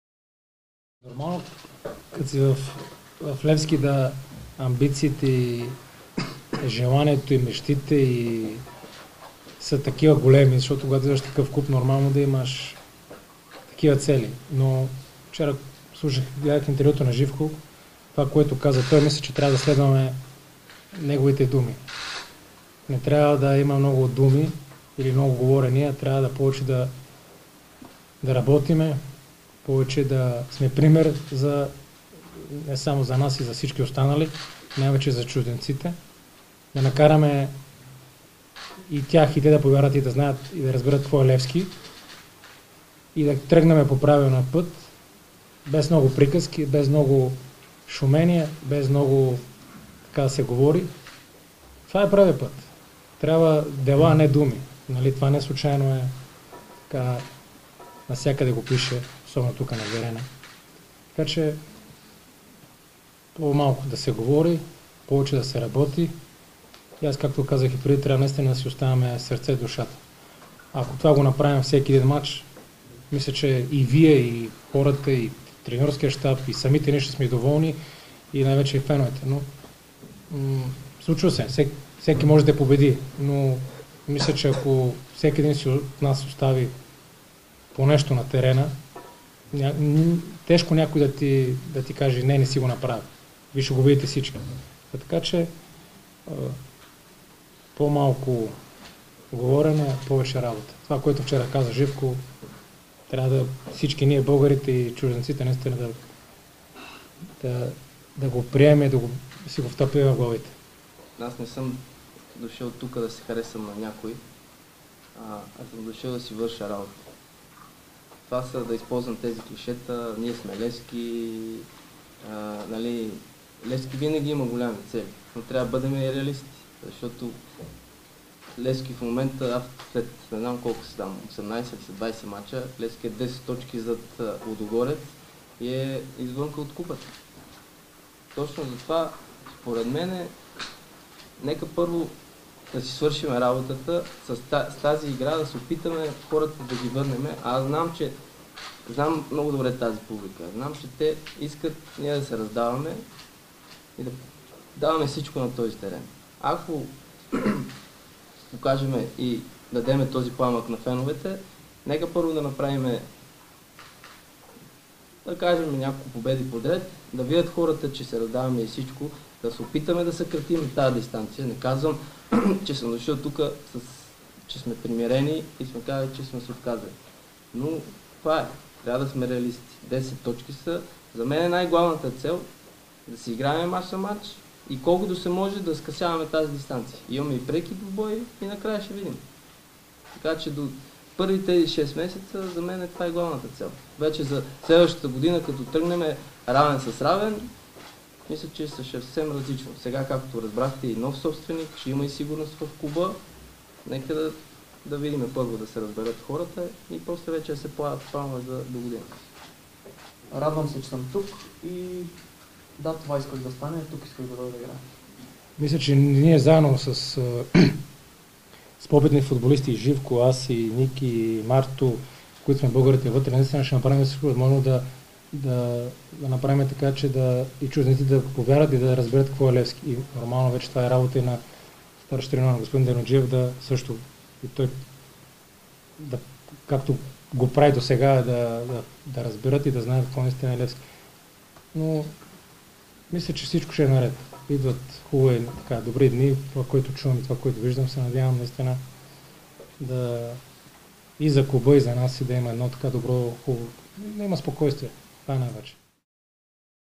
Новите в Левски: Трябват по-малко думи и повече дела
Трите нови попълнения на Левски Валери Божинов, Мартин Райнов и Живко Миланов бяха официално представени заедно с треньора Георги Дерменджиев. Играчите се обединиха около мнението, че не трябва да се изхвърлят с гръмки изказвания, а да работят упорито всеки ден и с делата си на терена да върнат обратно "синята" публика.

"Нормално е да има високи цели, когато си в Левски, но ние трябва да заложим на повече работа, а не толкова на приказките. Отборът е добър: Кабрал, Паулиньо, Тиам, Нуно Рейш, Цветкович, Яблонски са все качествени играчи. Смятам, че идват добри дни", заяви Божинов пред медиите. /БГНЕС